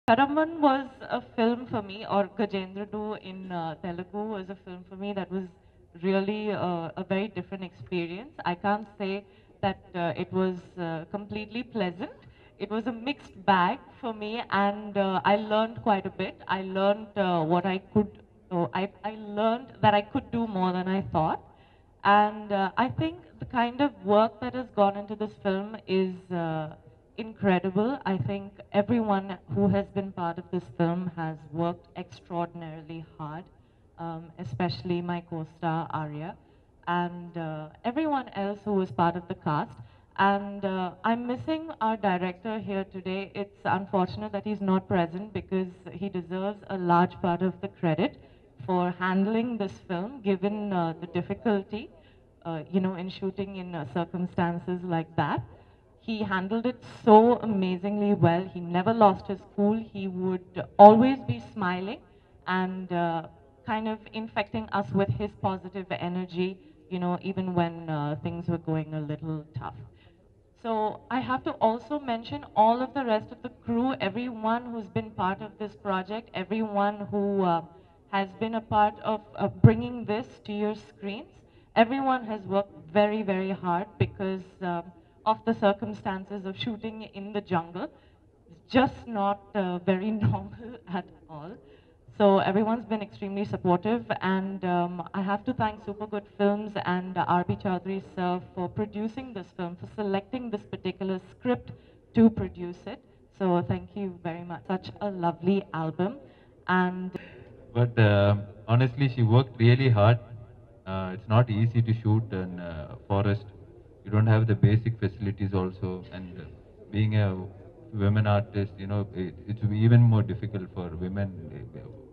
Catherine Tresa Speech at Gajendrudu Movie Audio Launch || Arya, Catherine Tresa
Watch Catherine Tresa Speech at Gajendrudu Movie Audio Launch.